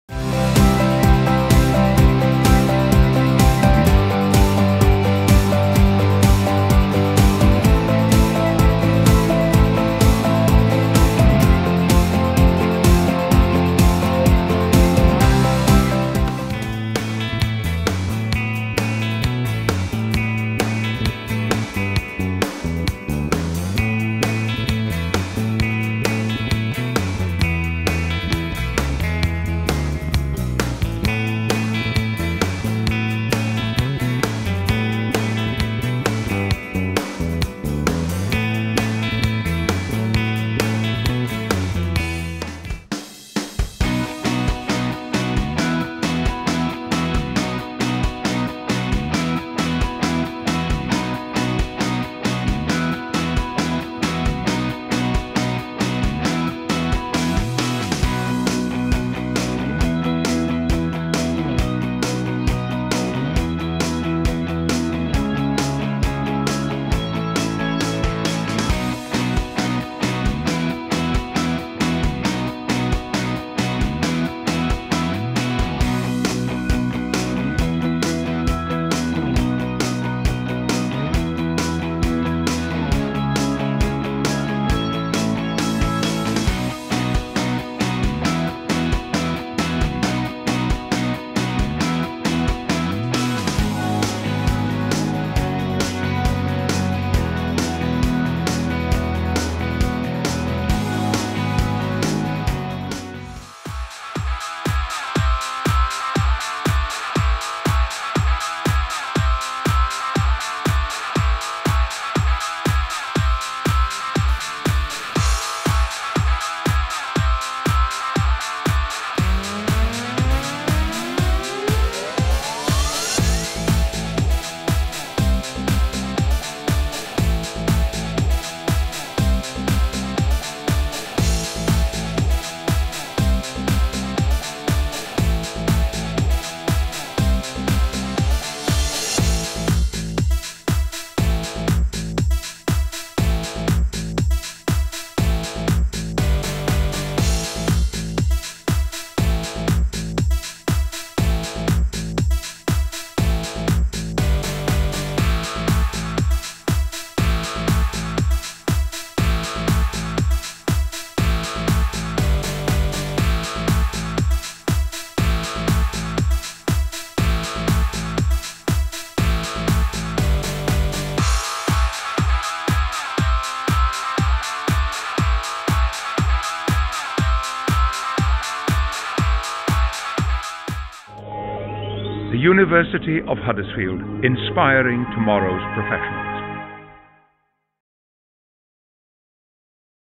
University of Huddersfield Events: EVENTS
University of Huddersfield Events Management Course, the event was hosted by the Students on the course: 'Events'. The Sequence was created for the Events Management Course.
Director, Editor and Camera Operator: Hollie Fairbairn
Camera Operator: Alex Bradley-Stocks